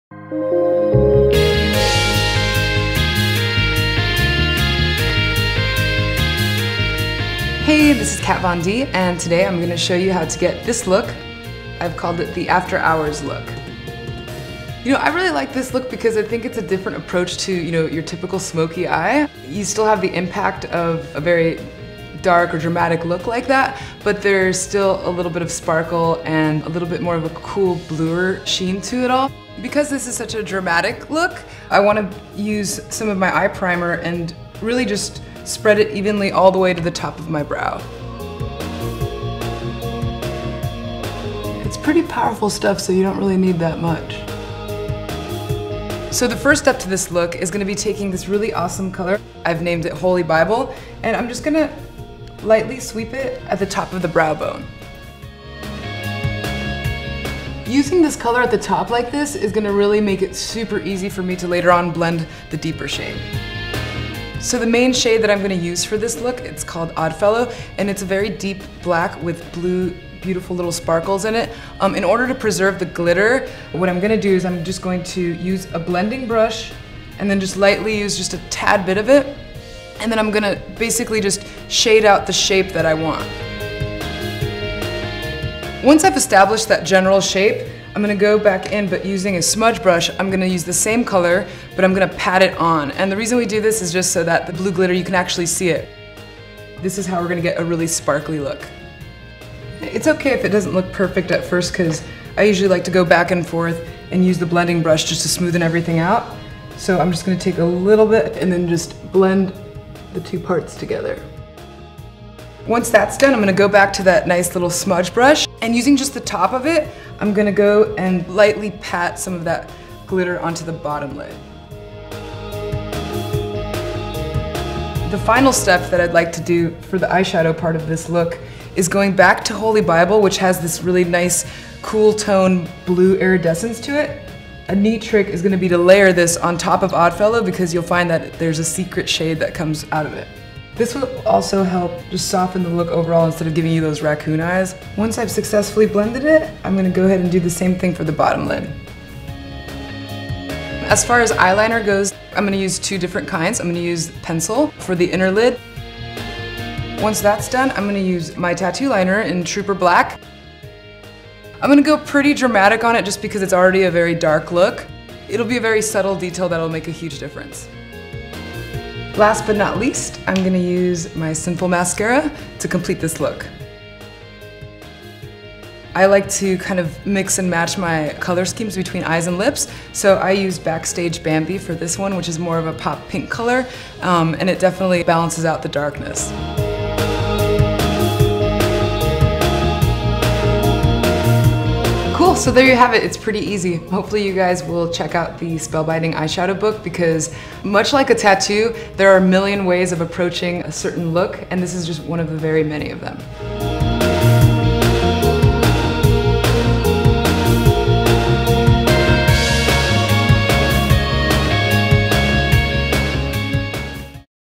Kat Von D Makeup - How To Get The After Hours Smoky Eye Look
Learn How To Get Kat Von D's Smoky Eye "After Hours" Look. Kat Von D Makeup Tutorial

How to get this look:
Step 1: Apply High Voltage Eye Primer all over the eyelid
Step 2: Sweep Spellbinding Eyeshadow in Holy Bible along the brow bone
Step 3: Apply Spellbinding Eyeshadow in Oddfellow to crease to build shape
Step 4: Pat on Spellbinding Eyeshadow in Oddfellow on the eyelid with a smudge brush to prevent fall out
Tip: Use a blending brush to make sure the shadow is evenly blended
Step 5: Use smudge brush to pat on Oddfellow along the bottom lid
Step 6: Layer on Holy Bible once more along the brow bone, blending into the darker shade to soften the look
Step 7: Add Waterproof Autograph Pencil Eyeliner in Puro Amor to the lid-rim
Step 8: Apply Tattoo Liner in Trooper Along the Upper Lashline
Step 9: Finish the eye with Sin-Full Lash Mascara in Black
Tip: Choose contrasting colors for your eyes and lips
Step 10: Pair the look with Backstage Bambi